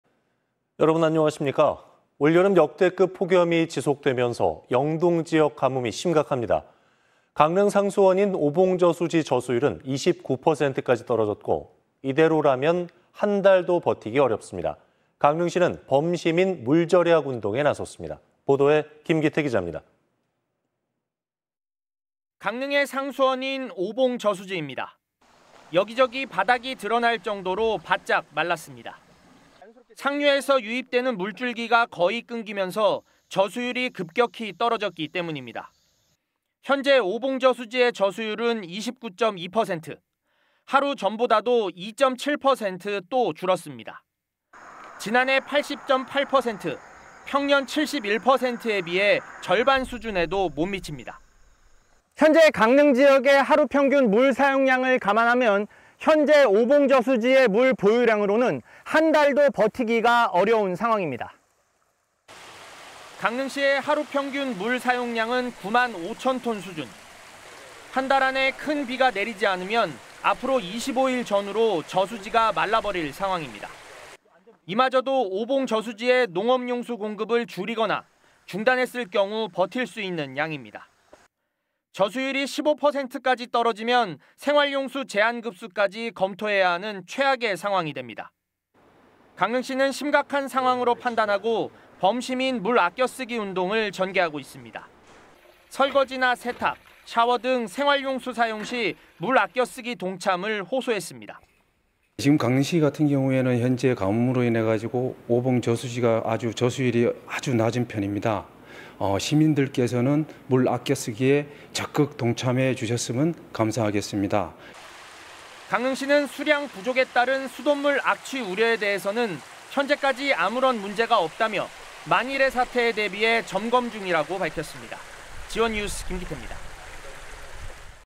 [G1뉴스]여름 가뭄 비상..제한급수 초읽기
올 여름 역대급 폭염이 지속되면서 영동지역 가뭄이 심각합니다.

 강릉 상수원인 오봉저수지 저수율은 29%까지 떨어졌고,

 이대로라면 한달도 버티기 어렵습니다.

 강릉시는 범시민 물 절약 운동에 나섰습니다.